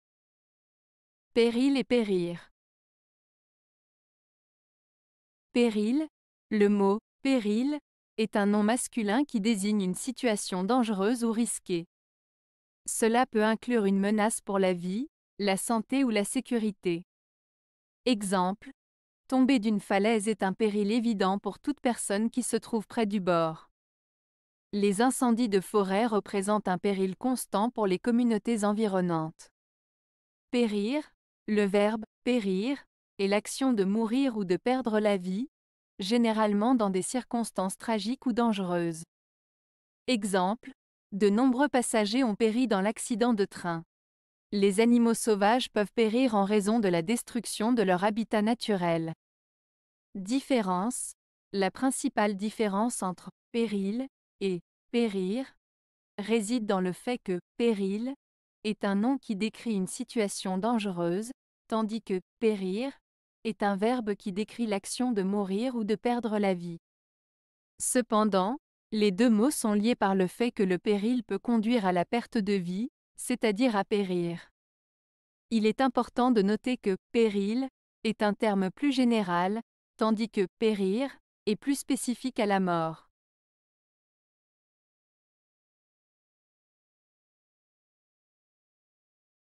Péril et Périr (Une Lettre)
Péril:
Le mot "péril" est un nom masculin qui désigne une situation dangereuse ou risquée. Cela peut inclure une menace pour la vie, la santé ou la sécurité.

Exemples:
- Tomber d'une falaise est un péril évident pour toute personne qui se trouve près du bord.
- Les incendies de forêt représentent un péril constant pour les communautés environnantes.

Périr:
Le verbe "périr" est l'action de mourir ou de perdre la vie, généralement dans des circonstances tragiques ou dangereuses.

Exemples:
- De nombreux passagers ont péri dans l'accident de train.
- Les animaux sauvages peuvent périr en raison de la destruction de leur habitat naturel.

Différence:

La principale différence entre "péril" et "périr" réside dans le fait que "péril" est un nom qui décrit une situation dangereuse, tandis que "périr" est un verbe qui décrit l'action de mourir ou de perdre la vie. Cependant, les deux mots sont liés par le fait que le péril peut conduire à la perte de vie, c'est-à-dire à périr. Il est important de noter que "péril" est un terme plus général, tandis que "périr" est plus spécifique à la mort.